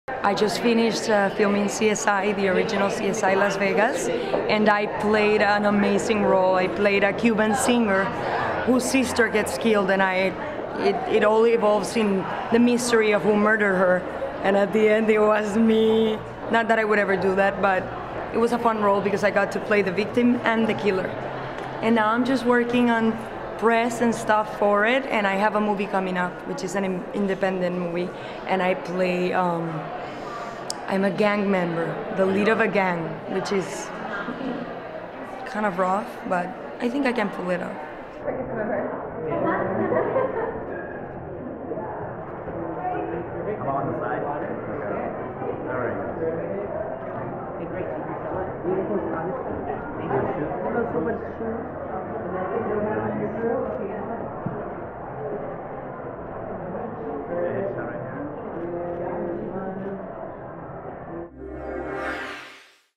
Sandra Vergara Plays Cuban Singer on CSI Las Vegas - Sofia Vergara's Sister!
Sandra Vergara (Adopted sister of Sofia Vergara, she is a Colombian actress and model as well as host) talks about playing a Cuban singer and assassin in CSI Las Vegas at Kathy Duliakas' 5th Annual Celebrity Oscar® Suite & Party benefiting the Kind Campaign at Taglyan Cultural Complex in Los Angeles, Ca USA February 21, 2013